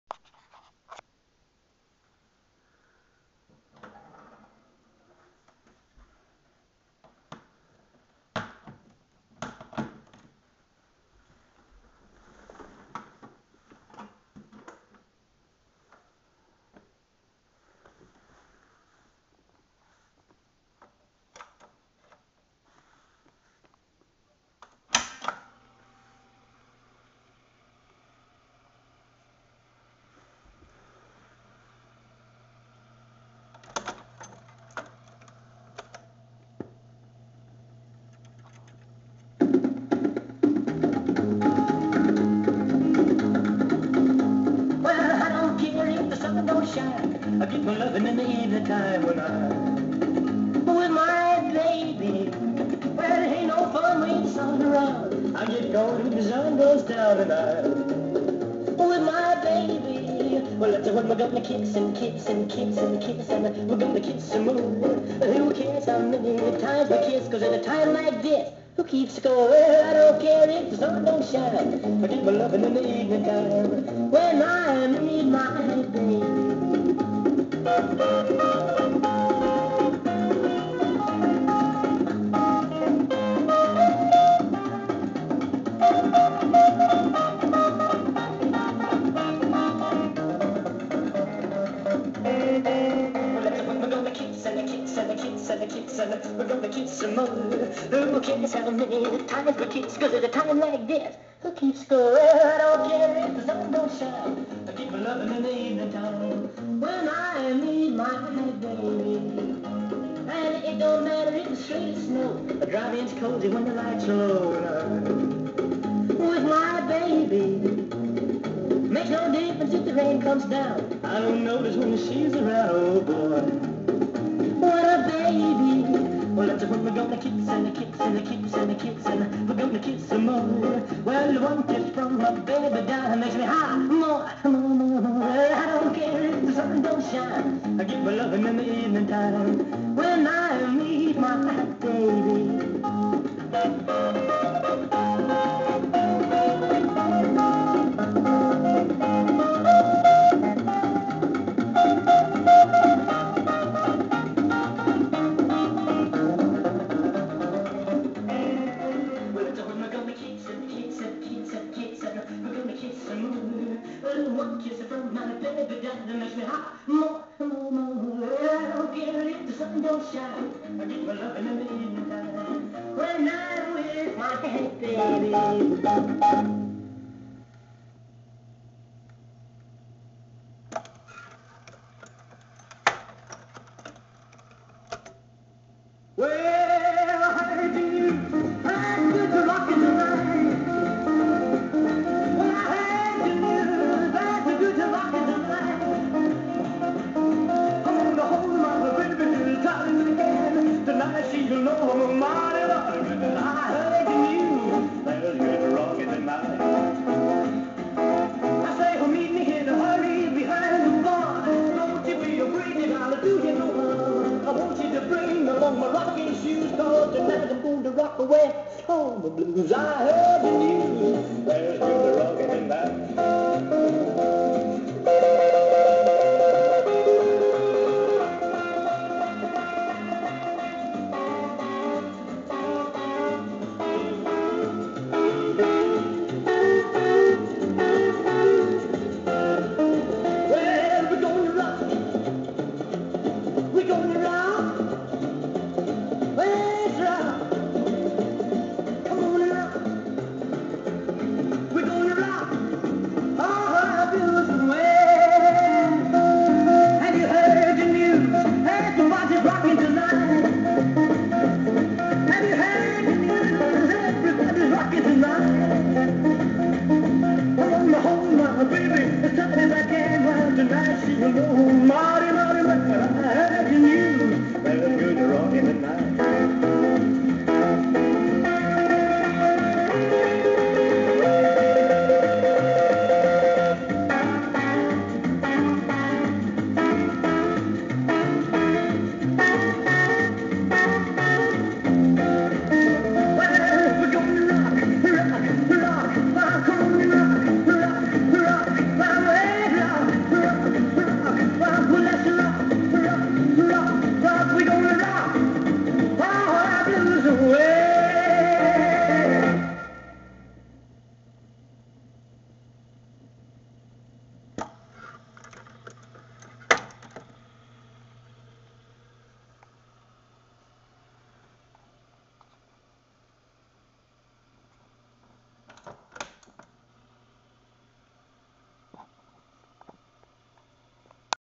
Discomatic Jukebox - The ipod of the 60's - JOHN LENNON'S JUKEBOX
This is a better quality video for the Discomatic Jukebox.  Famous because John Lenon carried it around to play his records while on tour.  Has Internal speaker and amplifier.  40 record capacity.  Plays both sides.